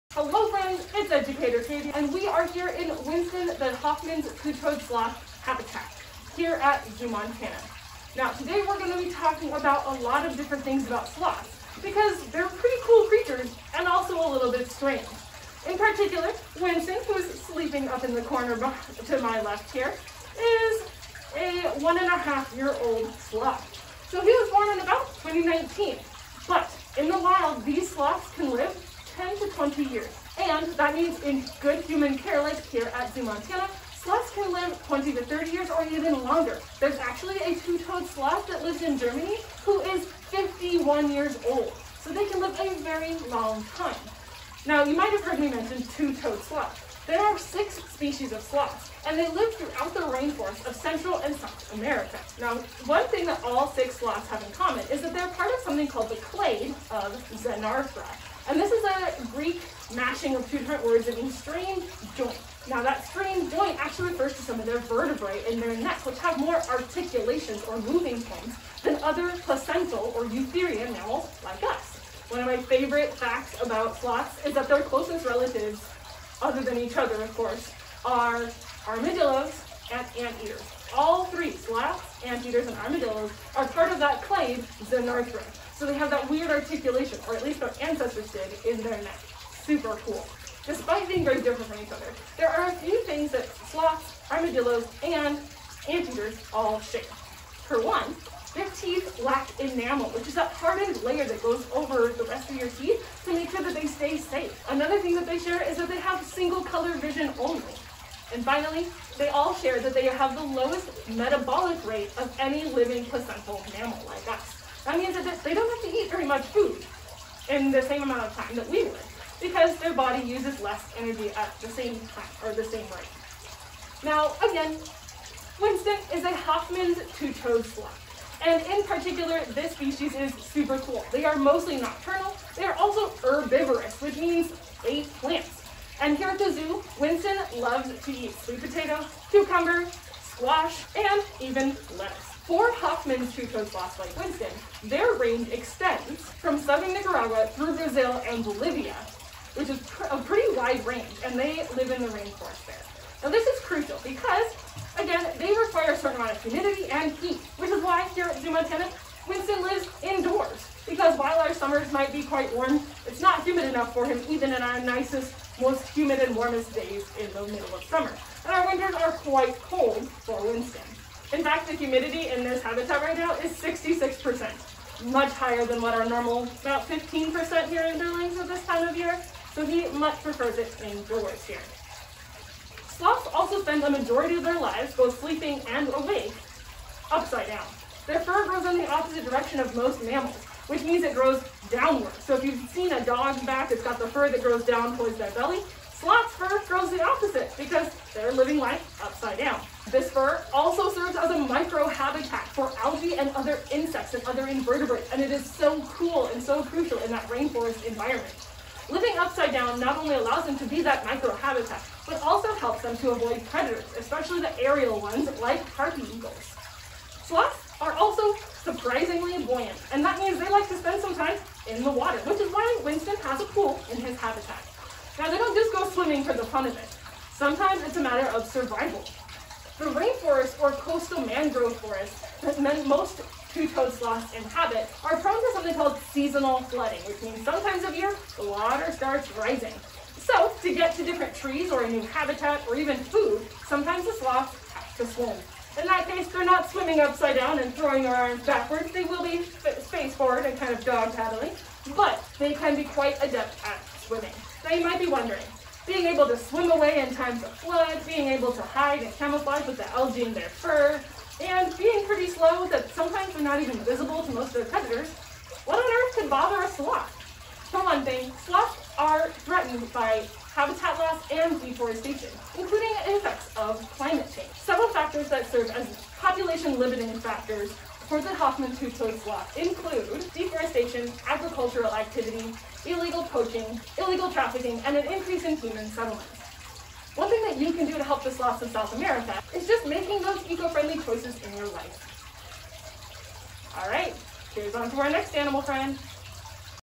Greybull Public Library SRP: Hoffman's two toed sloth encounter
Join Educator Katie and Winston, the Hoffman's two toed sloth, to learn all about these amazing animals and their rainforest homes for Greybull Library's Summer Reading program.